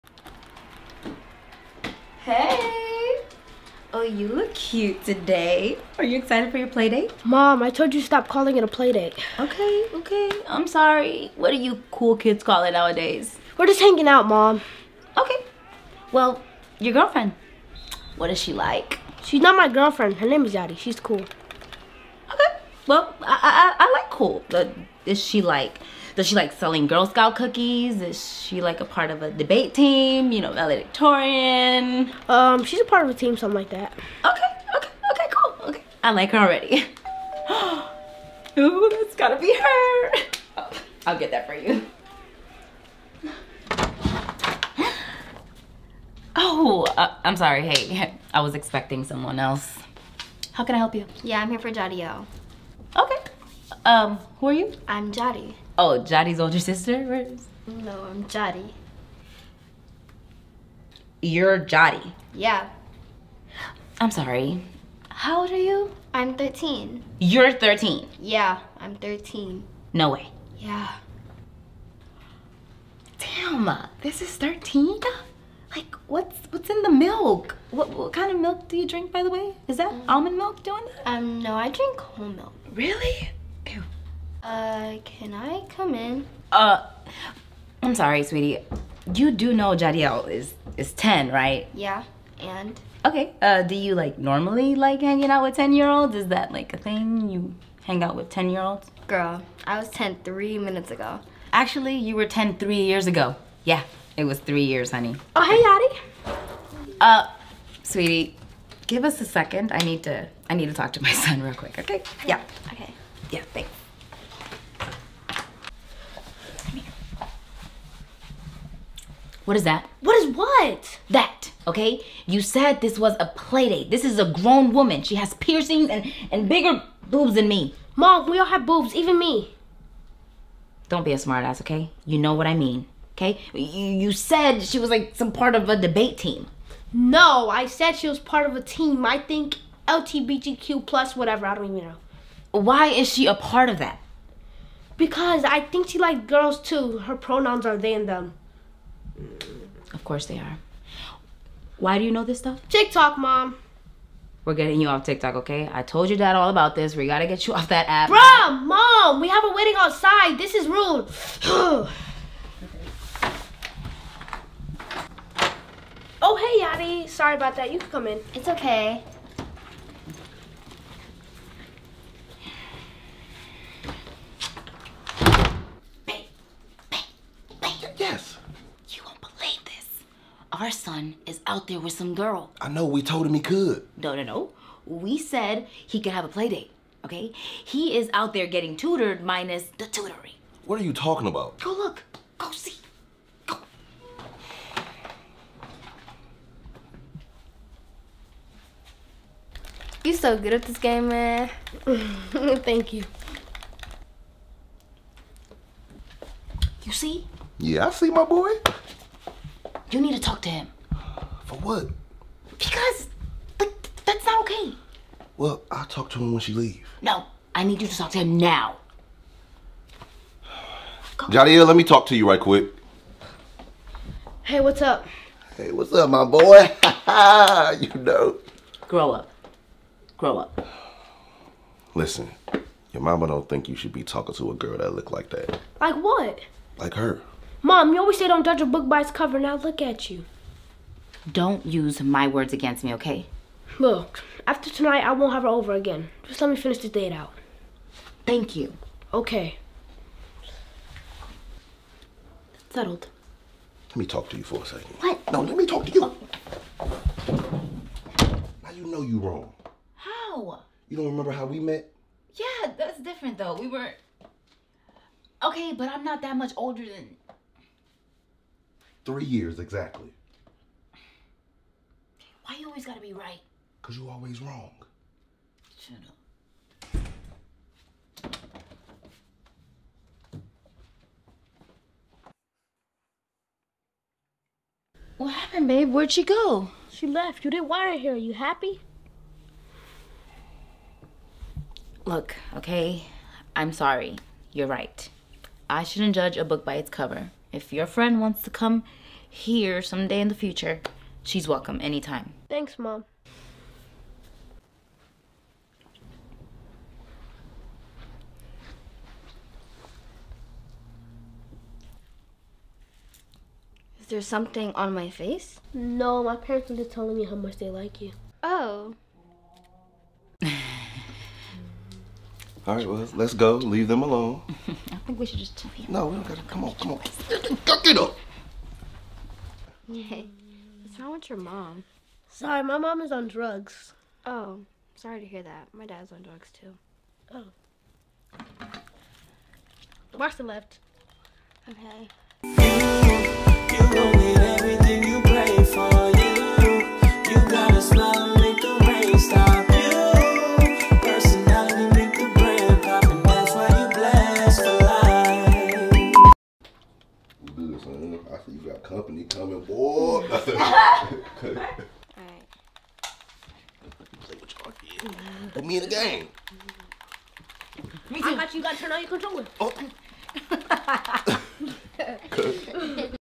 My son gotta "PLAYDATE"
Im not ready for my son to have playdates in real life !!!

Written by : ME @Efrangeliz

Shot/ edited by : @BlackEagleEntertainment
Starring :

Outro song by @eddiewhite9861

Outro by: @EeezyFilms

Should I  make more content with kids ??!

Thank you to all my loyal supporters , i   really love y'all !